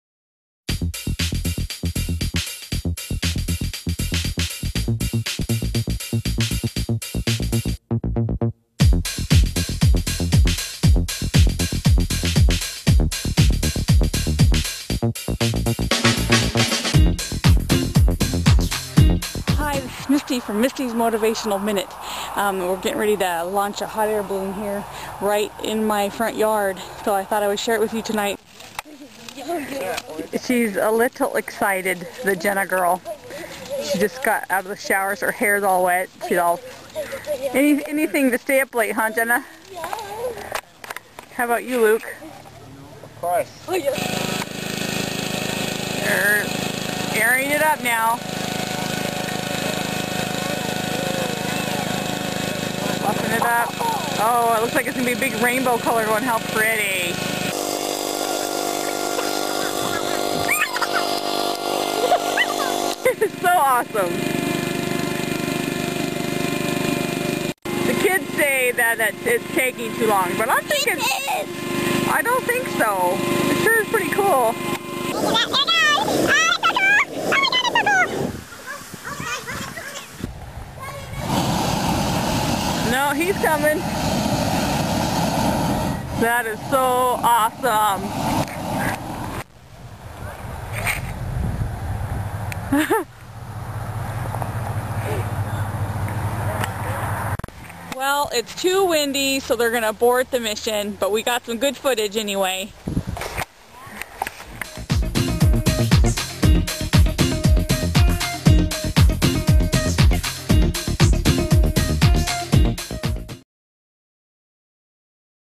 Full Of "HOT" Air
Surprise hot air balloon launch in Misty's front yard! So cool! Keeps Motivational juices flowing!